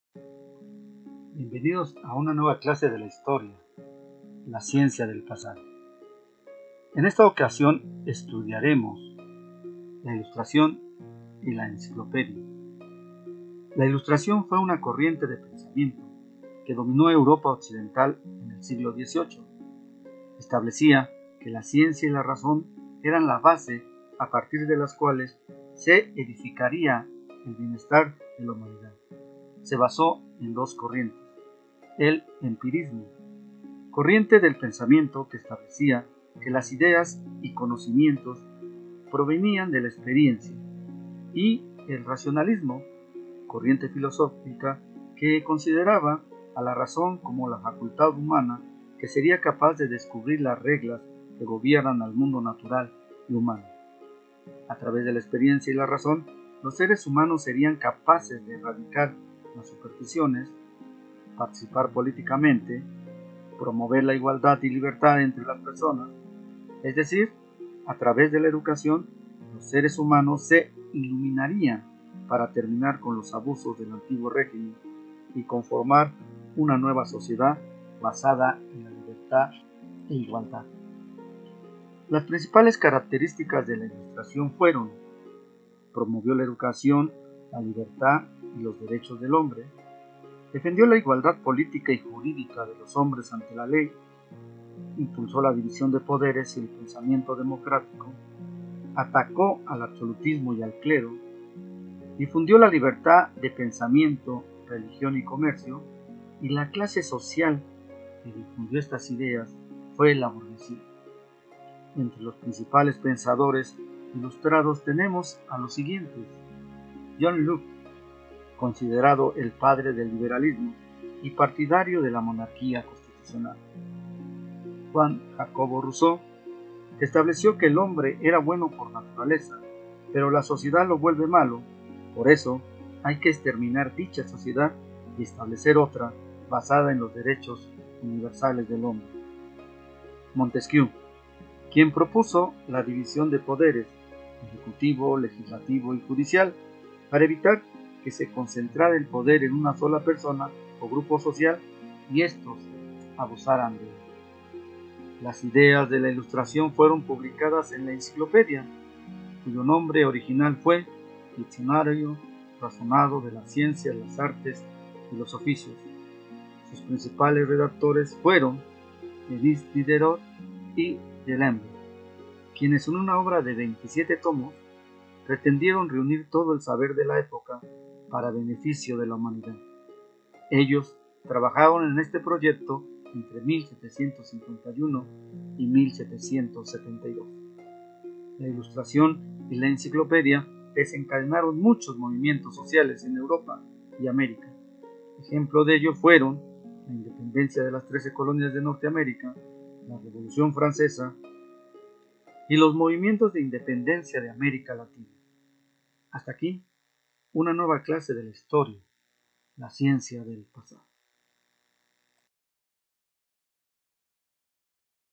2.1 Las nuevas ideas: La Ilustración y la Enciclopedia#comipems
La Ilustración representó una revolución en las ideas del siglo XVIII. Atacó al antiguo régimen, a través, de las ideas de igualdad y libertad.